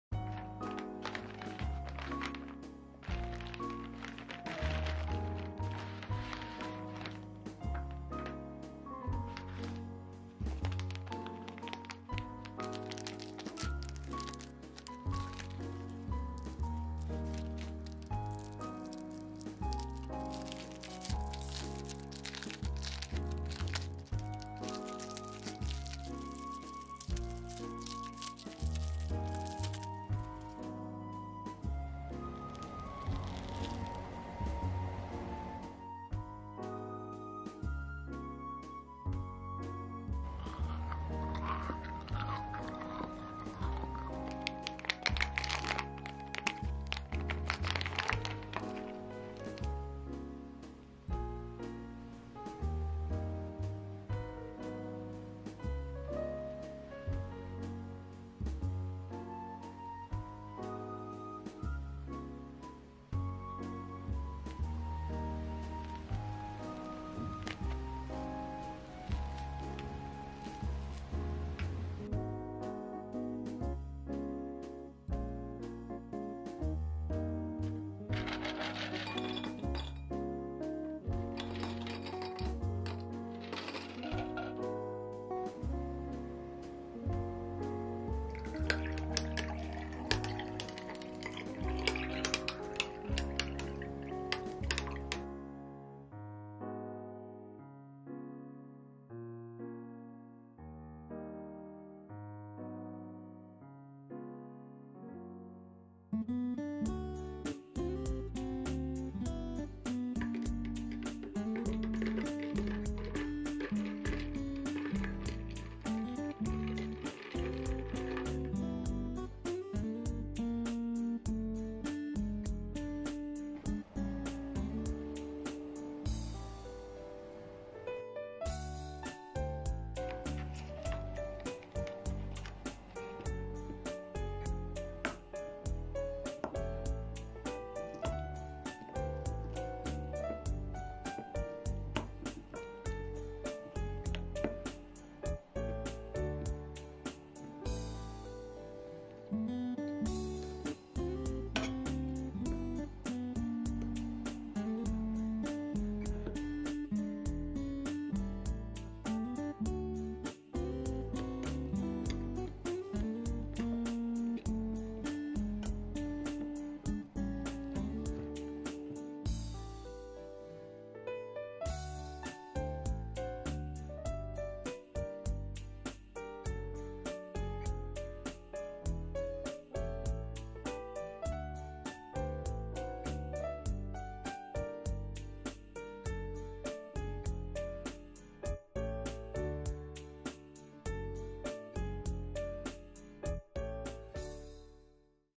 장인한과 약과 드디어 먹어봤어요! 거기에 홈카페를 곁들인..
안녕하세요 여름테이블입니다😊

아직까지도 사기 힘든 장인한과의 약과
저도 약켓팅을 뚫고 먹어볼 수 있었는데요~
저는 장인한과 납품처인 "별미담" 이라는 곳에서 구매했어요!

먹어보니 왜 이렇게 인기인지 알 수 있는 맛이더라구요~

근데 언제든지 쉽게 살 수 있는게 아니라서 그런지
저는 한 번 먹어본걸로 만족하려구요ㅋㅋ

올라오는 영상들을 조금 더 빠르게 확인하고 싶다면
인스타도 놀러와주세요⬇👀✨⬇

여름테이블 yeorumtable 에 있습니다.